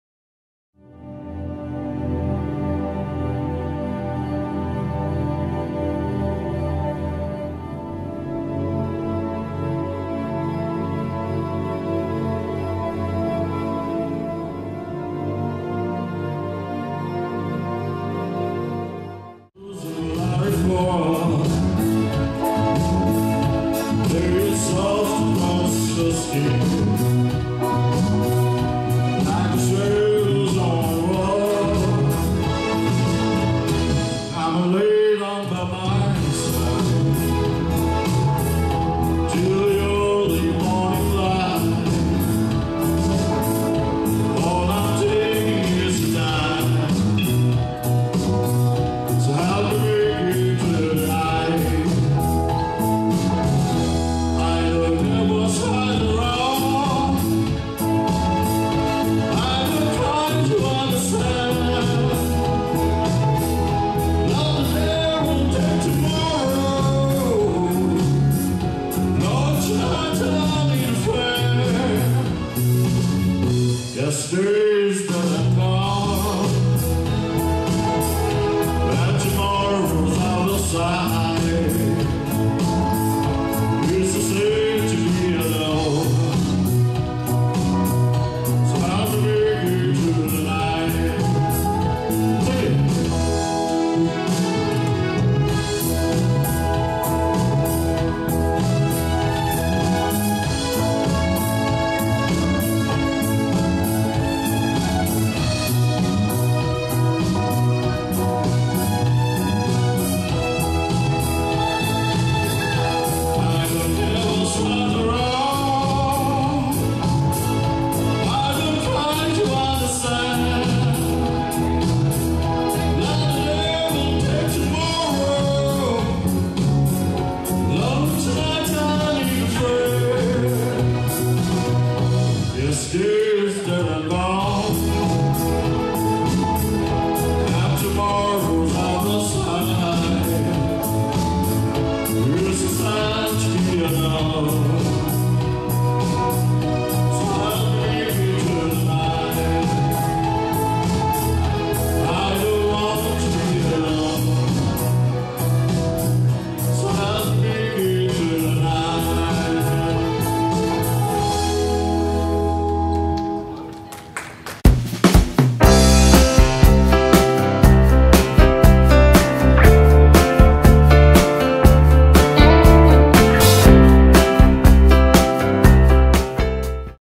Goan Summer Fiesta 2018 Swindon uk
GOAN FOLKTALES UK present Goan Summer Fiesta 2018 at Sikh Community Centre Swindon.